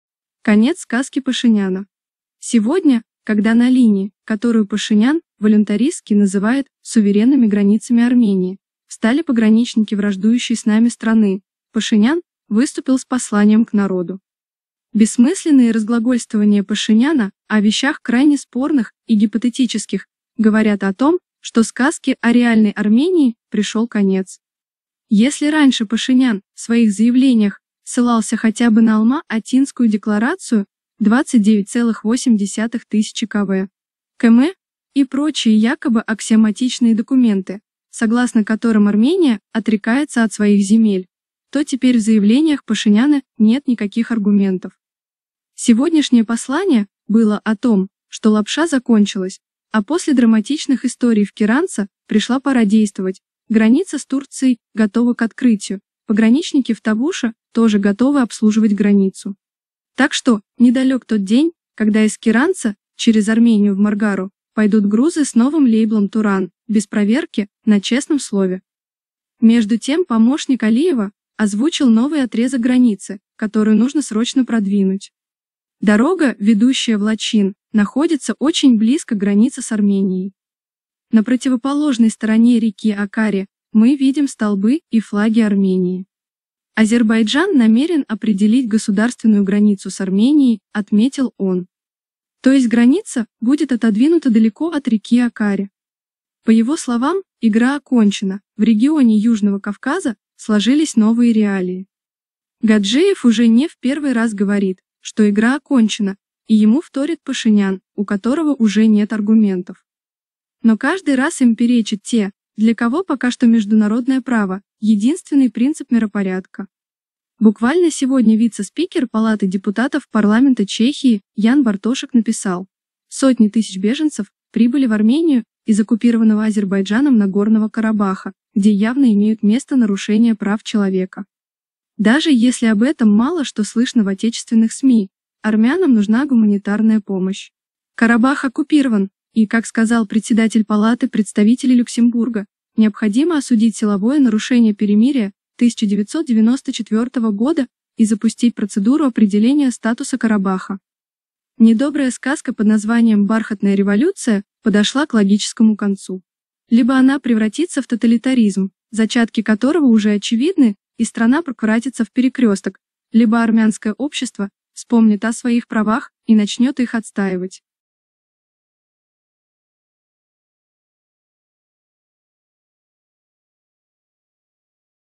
Конец сказки Пашиняна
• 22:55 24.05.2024

Сегодня, когда на линии, которую Пашинян волюнтаристски называет «суверенными границами» Армении, встали пограничники враждующей с нами страны, Пашинян выступил с посланием к народу.
Бессмысленные разглагольствования Пашиняна о вещах крайне спорных и гипотетических говорят о том, что сказке о «реальной Армении» пришел конец. Если раньше Пашинян в своих заявлениях ссылался хотя бы на Алма-Атинскую декларацию, 29,8 тыс. кв.км и прочие якобы аксиоматичные документы, согласно которым Армения отрекается от своих земель, то теперь в заявлениях Пашиняна нет никаких аргументов.
Сегодняшнее послание было о том, что лапша закончилась, а после драматичных историй в Киранце пришла пора действовать — граница с Турцией готова к открытию, пограничники в Тавуше тоже готовы «обслуживать границу». Так что, недалек тот день, когда из Киранца через Армению в Маргару пойдут грузы с новым лейблом «туран» — без проверки, на «честном слове».
Между тем помощник Алиева озвучил новый отрезок «границы», которую нужно срочно продвинуть.
«Дорога, ведущая в Лачин, находится очень близко к границе с Арменией. На противоположной стороне реки Акари мы видим столбы и флаги Армении.  Азербайджан намерен определить государственную границу с Арменией», –отметил он. То есть граница будет отодвинута далеко от реки Акари.
По его словам, «игра окончена, в регионе Южного Кавказа сложились новые реалии».
Гаджиев уже не в первый раз говорит, что «игра окончена», и ему вторит Пашинян, у которого уже нет аргументов. Но каждый раз им перечат те, для кого пока что международное право — единственный принцип миропорядка.
Буквально сегодня вице-спикер Палаты депутатов парламента Чехии Ян Бартошек написал: «Сотни тысяч беженцев прибыли в Армению из оккупированного Азербайджаном Нагорного Карабаха, где явно имеют место нарушения прав человека. Даже если об этом мало что слышно в отечественных СМИ, армянам нужна гуманитарная помощь».
Карабах оккупирован, и, как сказал председатель Палаты представителей Люксембурга, необходимо осудить силовое нарушение перемирия 1994 года и запустить процедуру определения статуса Карабаха.
Недобрая сказка под названием «бархатная революция» подошла к логическому концу: либо она превратится в тоталитаризм (зачатки которого уже очевидны), и страна прквратится в «перекресток», либо армянское общество вспомнит о своих правах и начнет их отстаивать. Международное сообщество готово к этому. Все заявления Пашиняна о том, что «международное сообщество» требует «делимитации на основе советских карт» от лукавого — это предложения самого Пашиняна. И если будет другое предложение, международное сообщество готово будет его рассмотреть.

Геноцид Армян в Арцахе
Армения
Арцах
Карабах
 Karabakh
Азербайджан
Срочные новости

Экстренные новости
Новости дня
Վերջին լուրեր
Յութուբ լուրեր
Շտապ